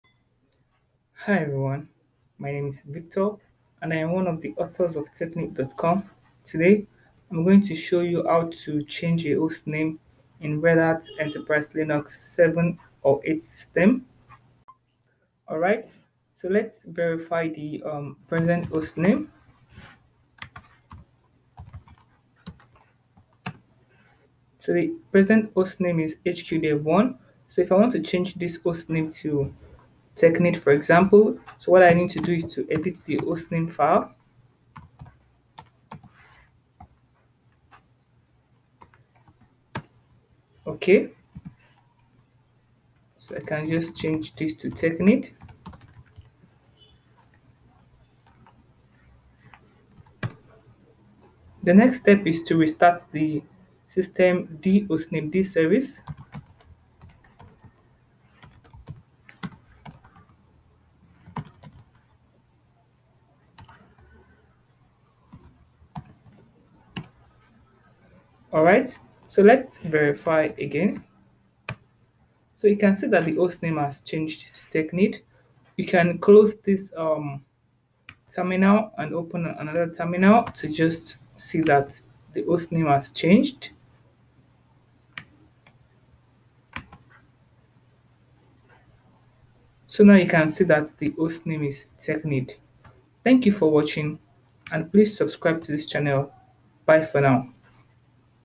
How To Change / Rename A Hostname In Linux - RHEL 7 & 8 (RHCSA 8, Lesson 9G)
see the step by step process of how to change / rename a hostname in Linux (RHEL / CentOS 7 & 8)